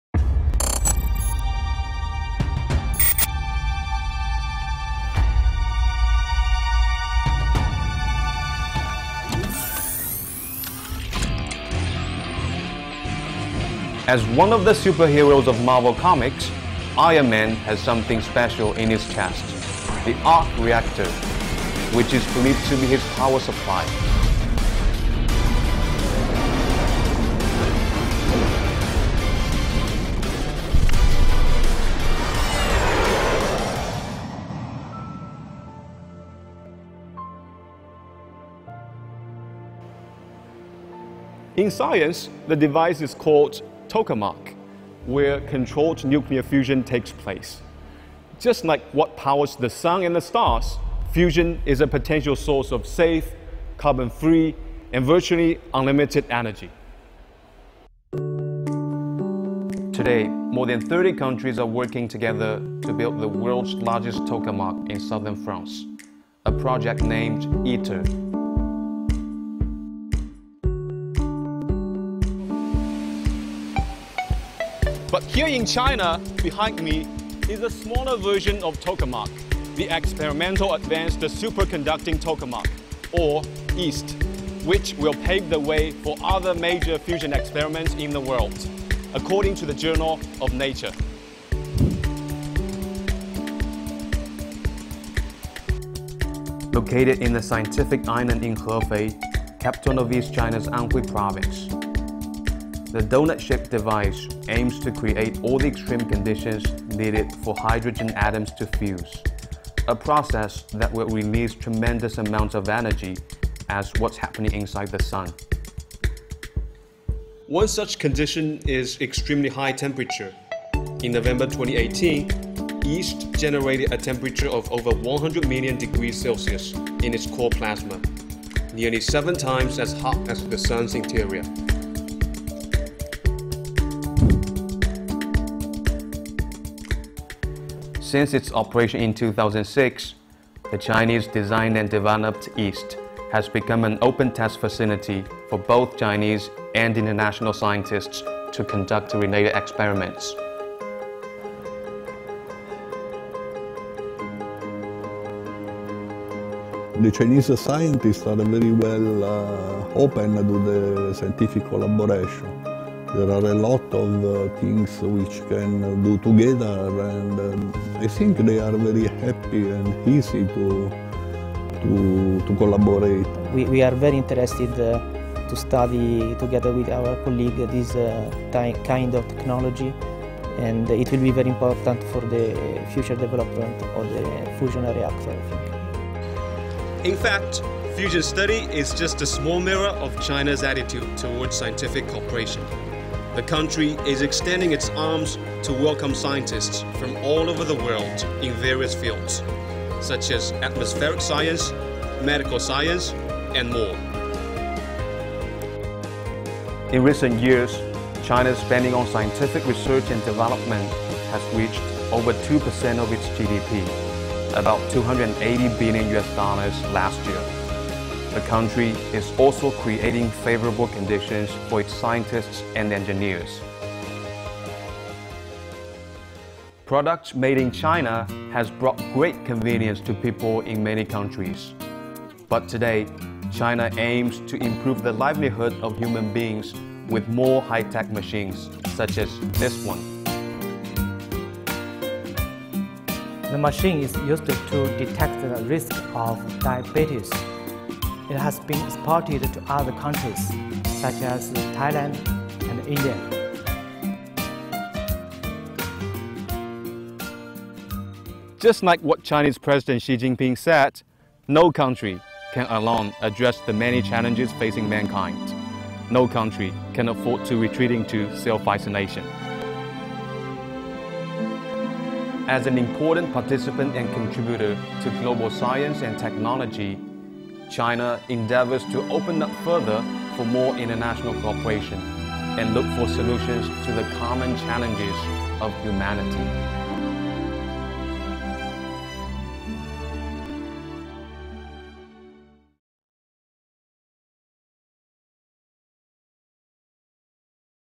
"Artificial sun": China in global cooperation for unlimited fusion energy
Fusion, the nuclear reaction that powers the Sun and the stars, is a potential source of safe, carbon-free, and virtually limitless energy. China is embracing and contributing to global scientific research on how to harness fusion energy